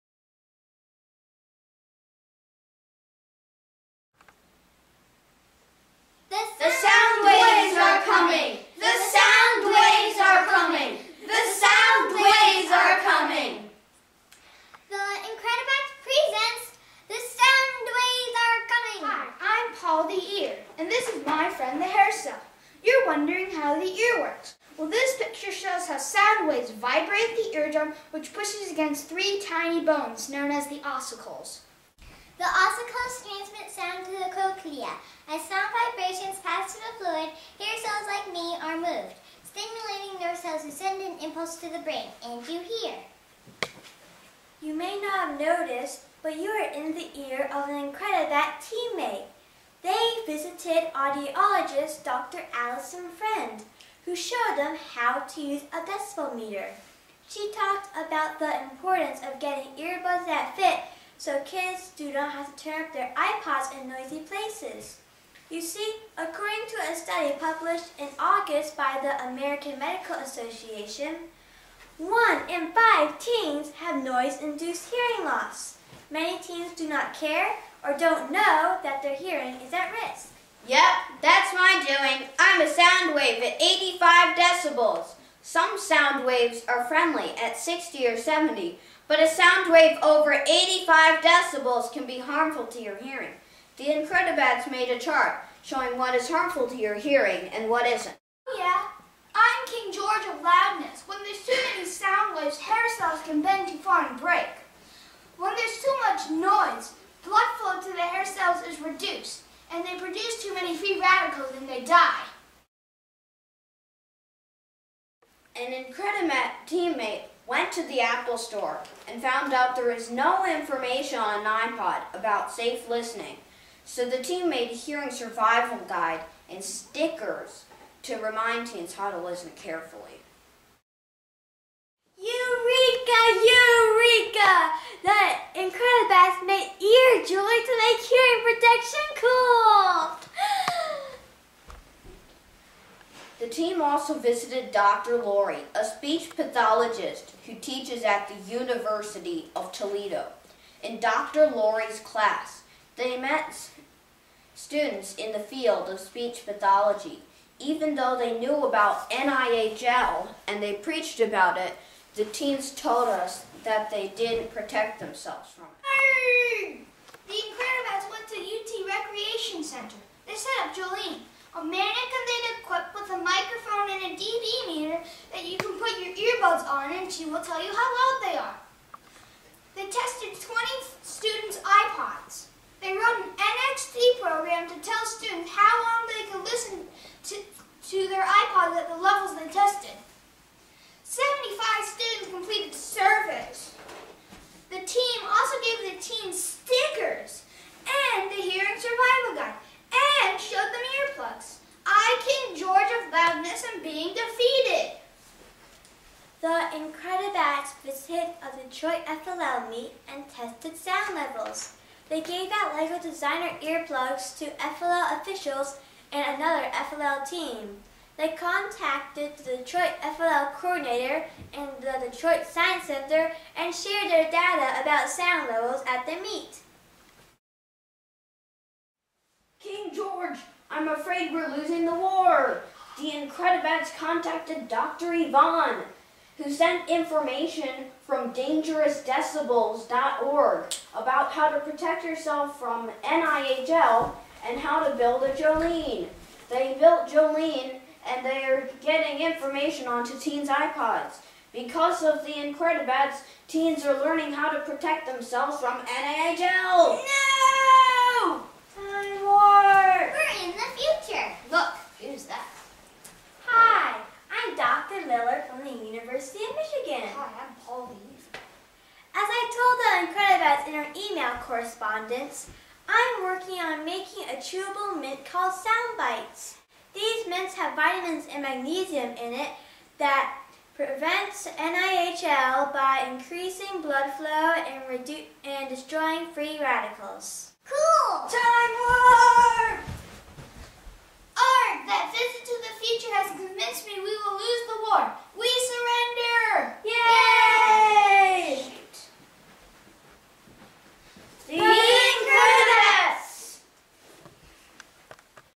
The Sound Waves are Coming: Preventing Noise-Induced Hearing Loss
2010 First Lego League Team #5772, The IncrediBATs, present "The Sound Waves are Coming,"  a skit about their research into noise-induced hearing loss.